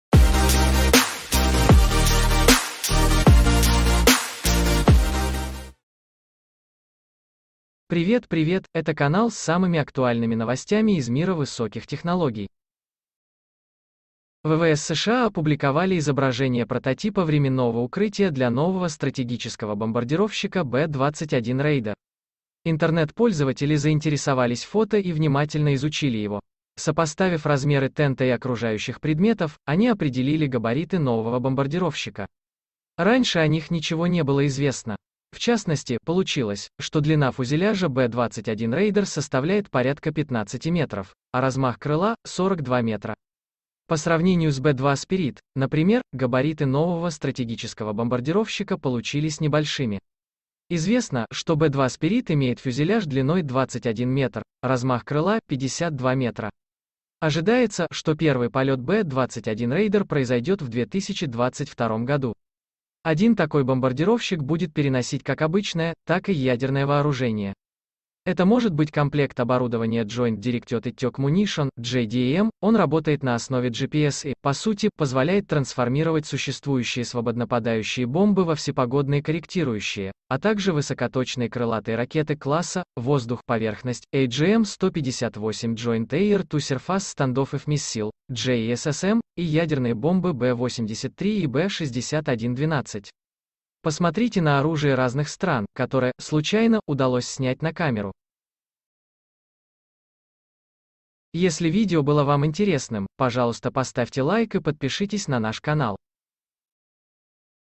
ВВС США случайно раскрыли данные о секретном бомбардировщике
По опубликованному в сети фото эксперты вычислили размеры B-21 Raider, которому пророчат стать самым крутым военным самолетом мира.